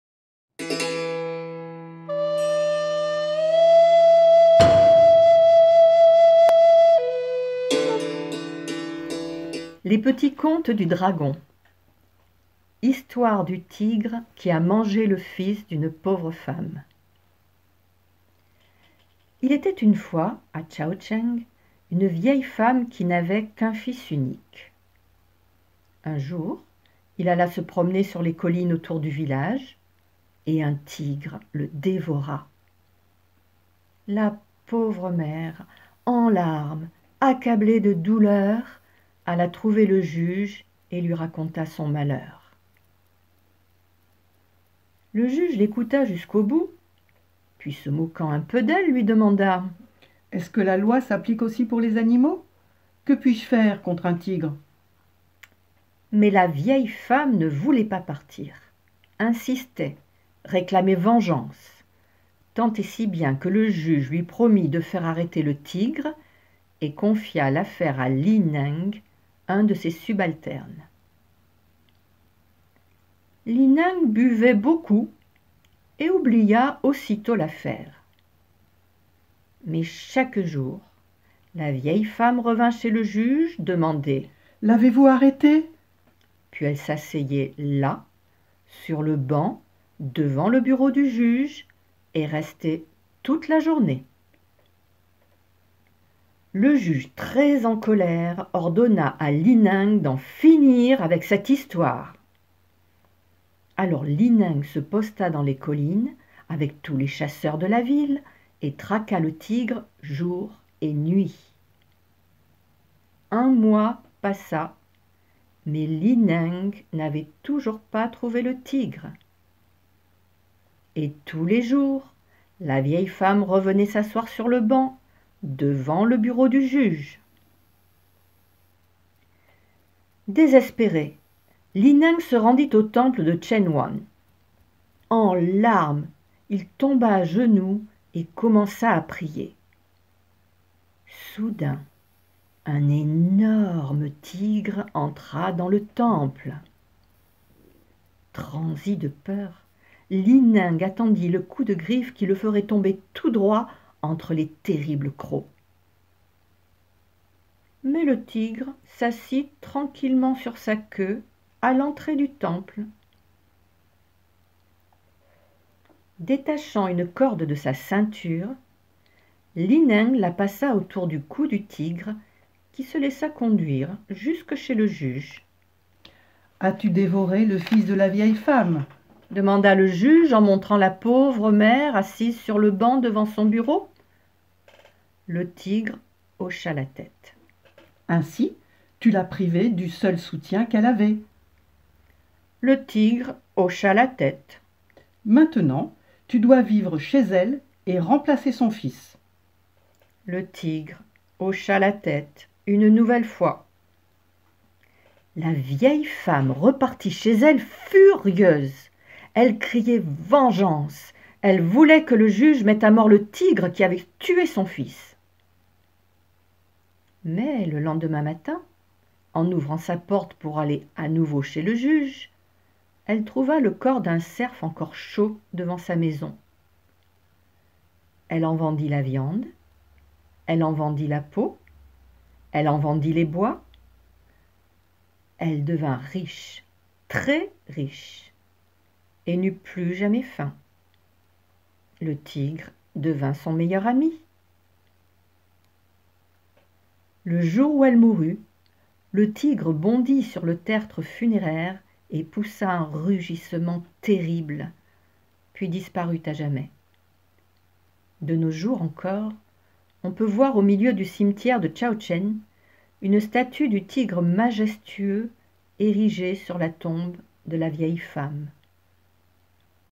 Les petits contes du dragon: L'histoire du tigre qui a mangé le fils d'une pauvre femme
Conte traditionnel chinois librement inspiré des Contes du Yin et du Yang.
Réalisation et manipulation des ombres: Nicole Perreau.
Conteuses: Claire Coudair, Fabienne Varoutsikos.

Un événement du festival du Nouvel An chinois organisé par Vents d'Asie.